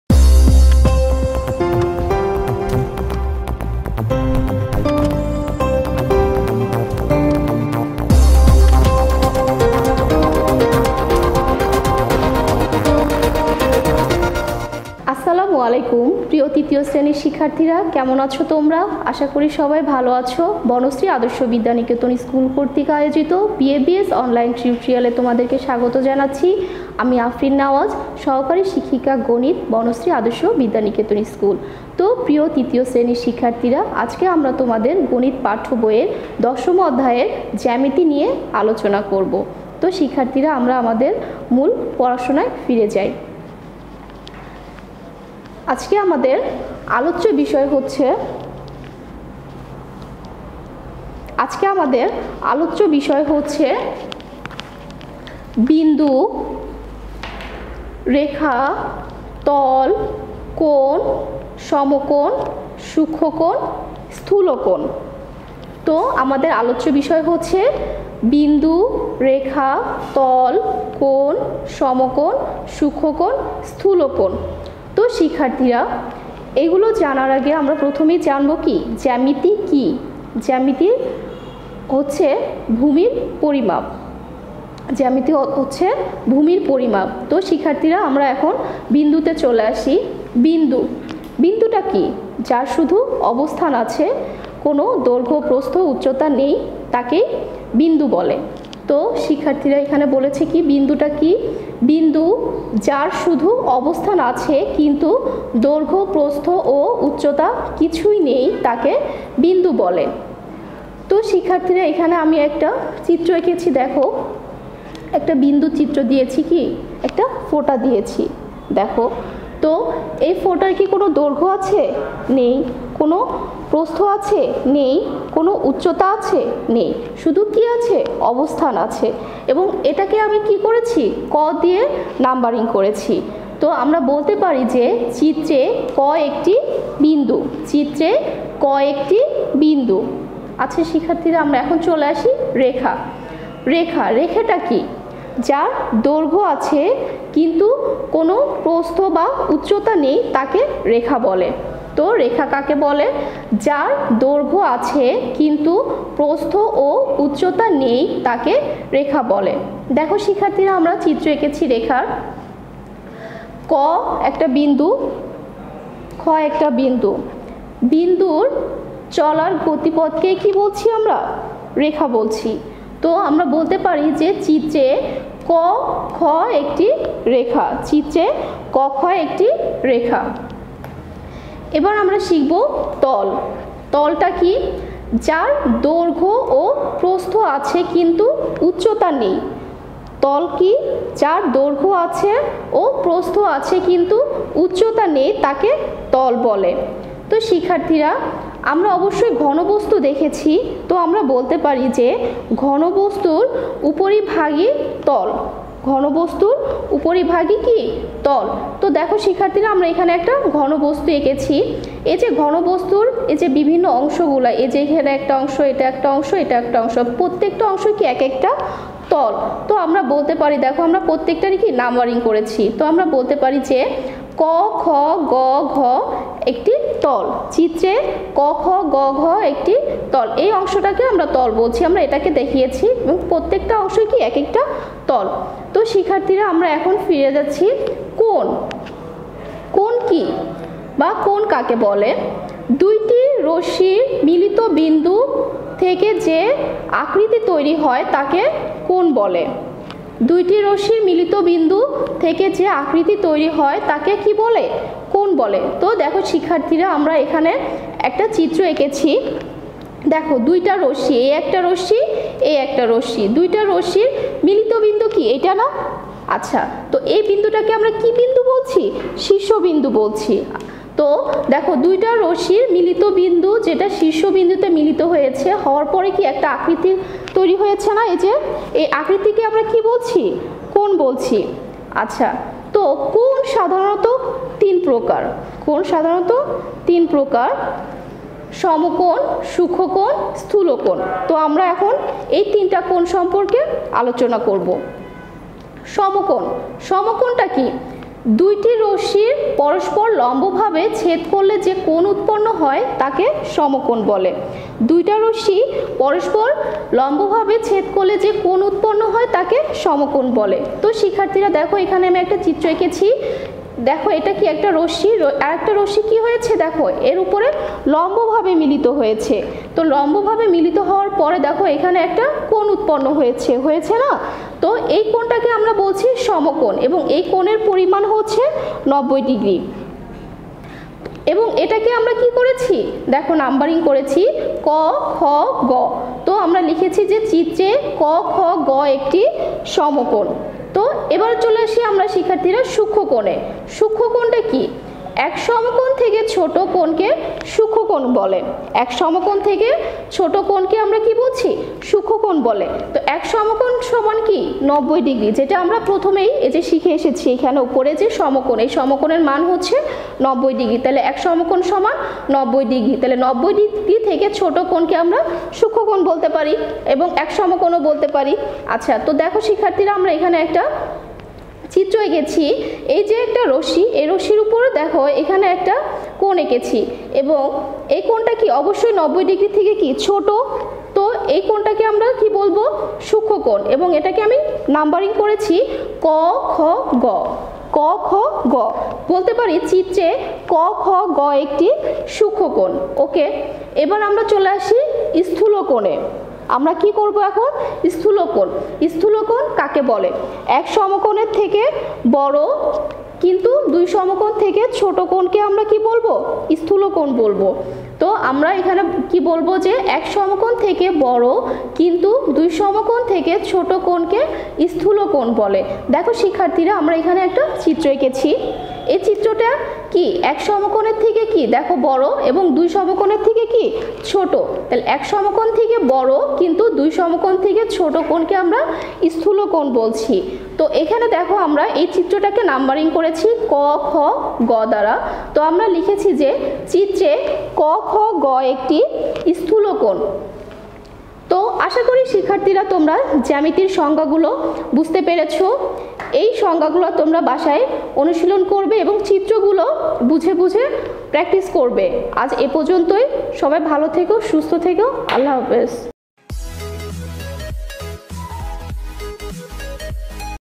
শ্রেণিঃ তৃতীয়, বিষয়ঃ প্রাথমিক গণিত, অধ্যায়ঃ জ্যামিতি, আলোচ্য বিষয়ঃ বিন্দু, রেখা, তল, কোণ, সমকোণ
বিষয় শিক্ষকঃ আফরিন নেওয়াজ